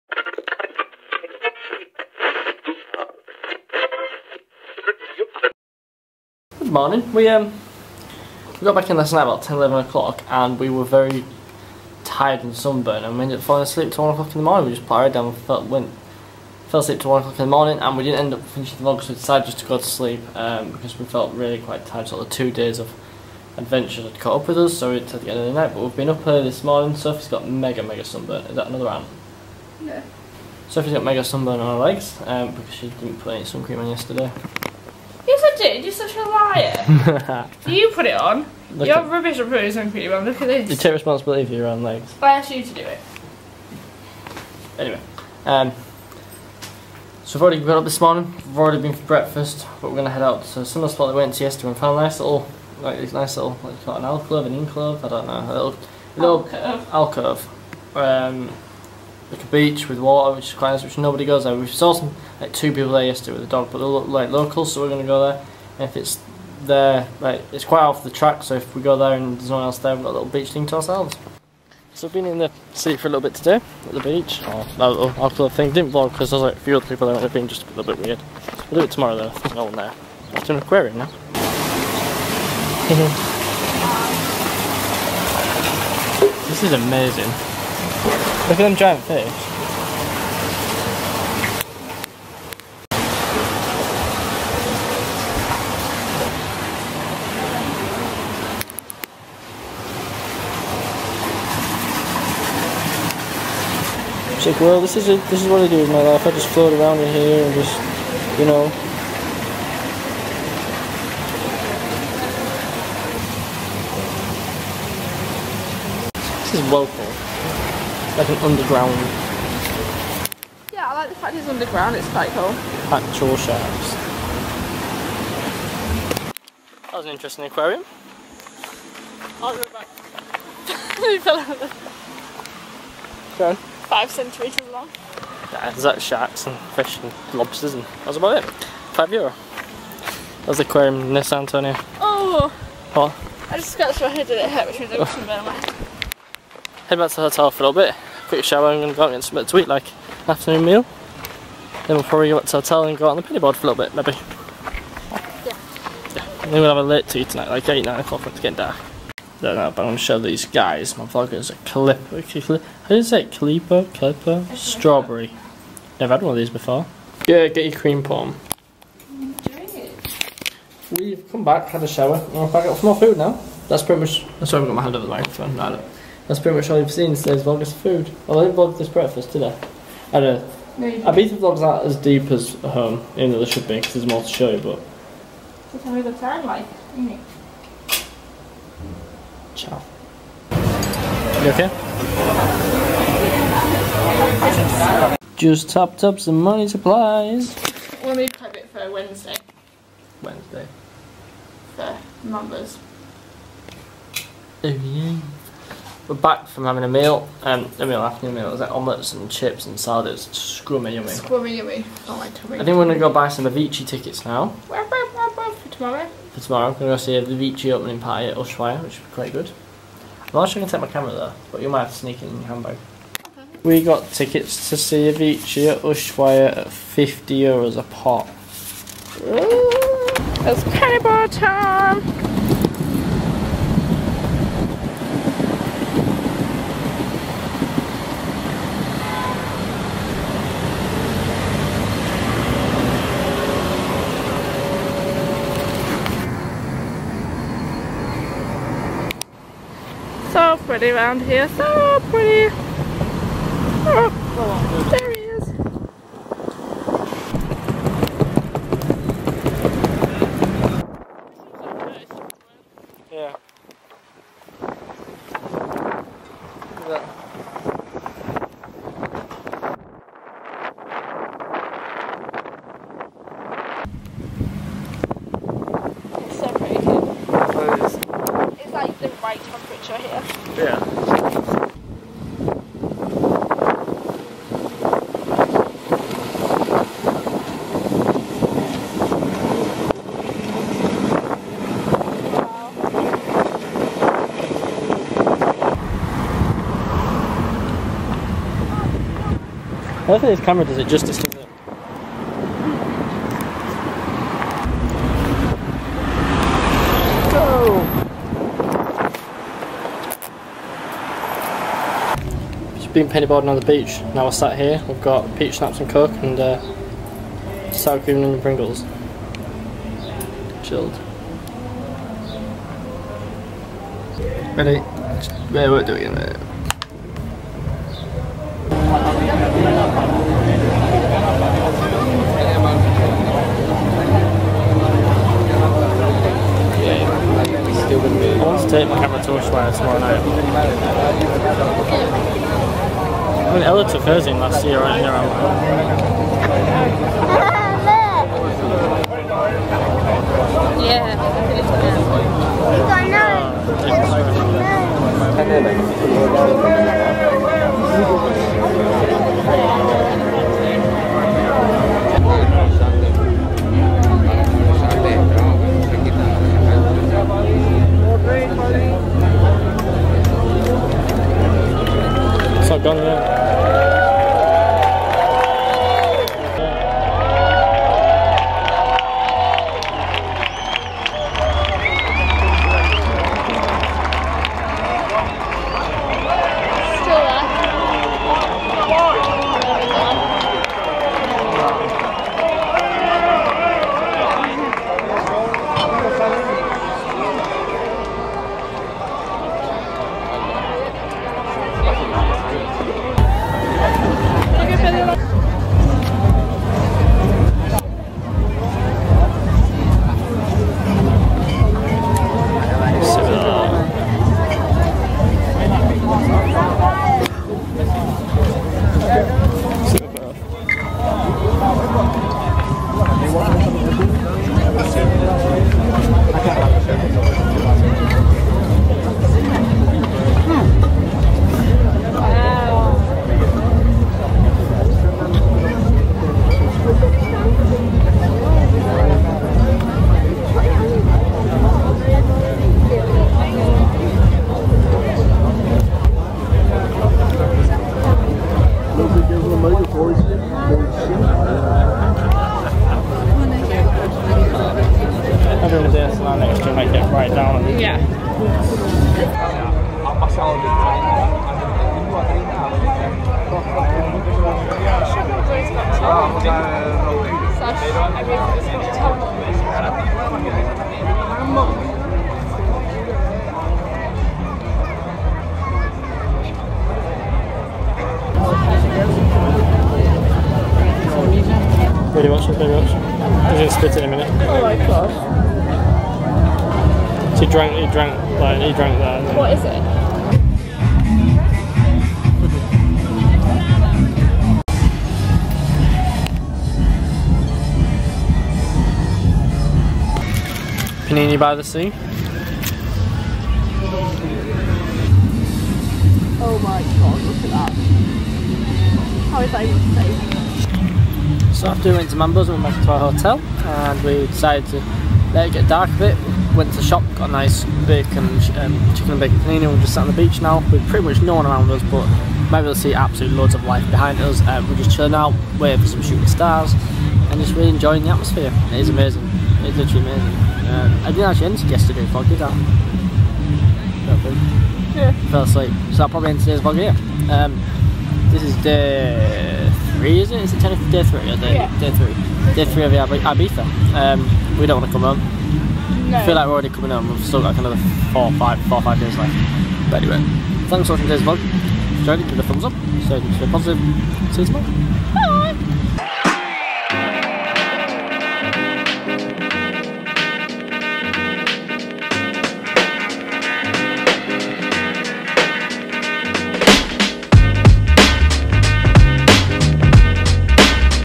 IBIZA DAY THREE - PENNY BOARDING IN IBIZA! | ItsJamieIRL Daily Vlogs
► July 11th 2015  - #957
- So happy I took my Penny Board to ibiza such a happy place to ride around and smooth roads!